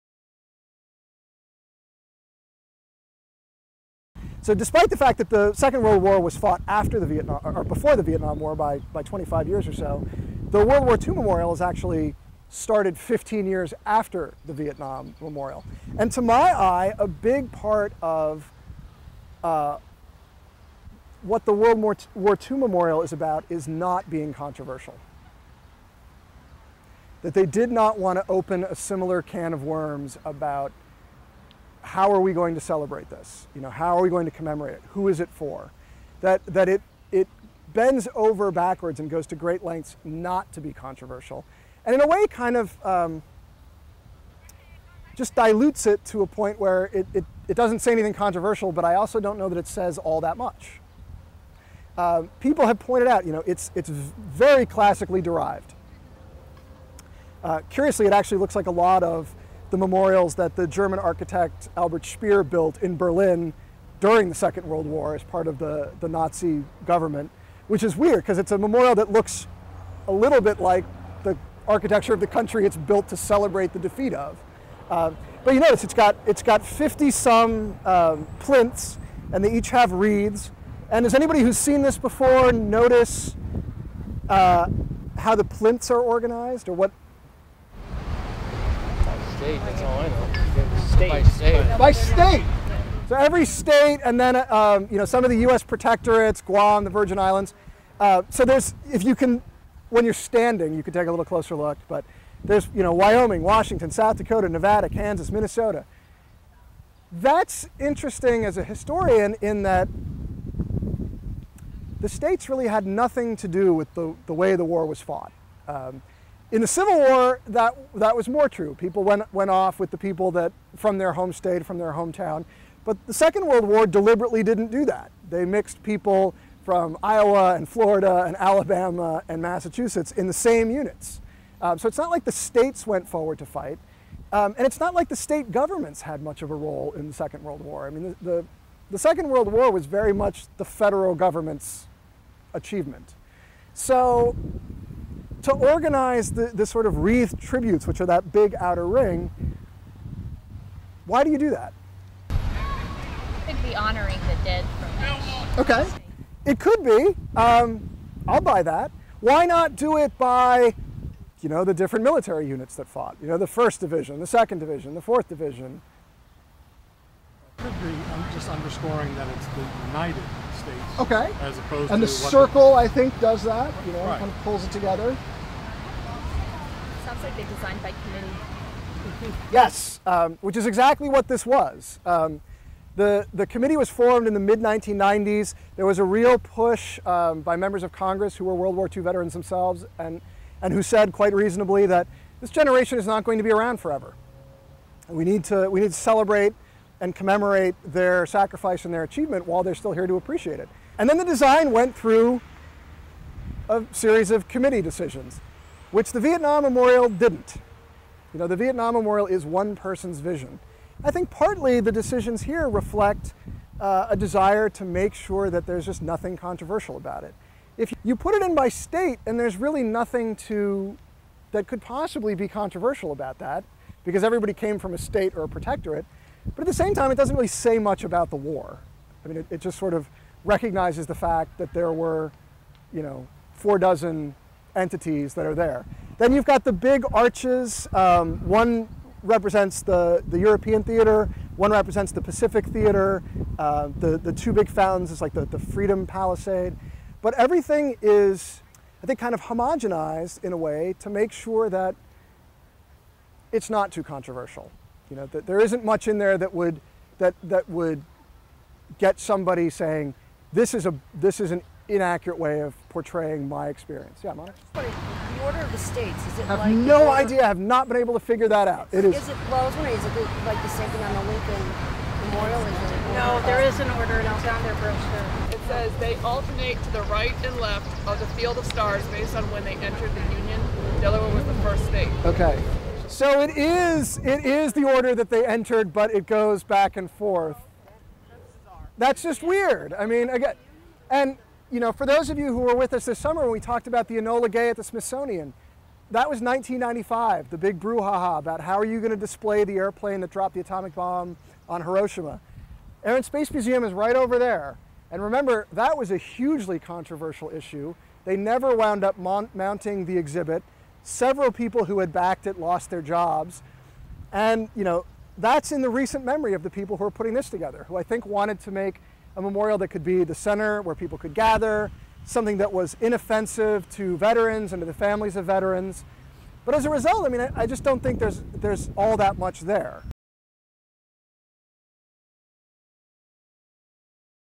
Project Spotlight - A Close Look at the World War II Memorial - The WWII Memorial in Contrast
Historian Christopher Hamner introduces educators to the World War II Memorial in Washington, DC. He places the memorial in context: How does the story of its construction contrast with that of the Vietnam Veterans Memorial? Who was the World War II Memorial built for? What was its design intended to convey?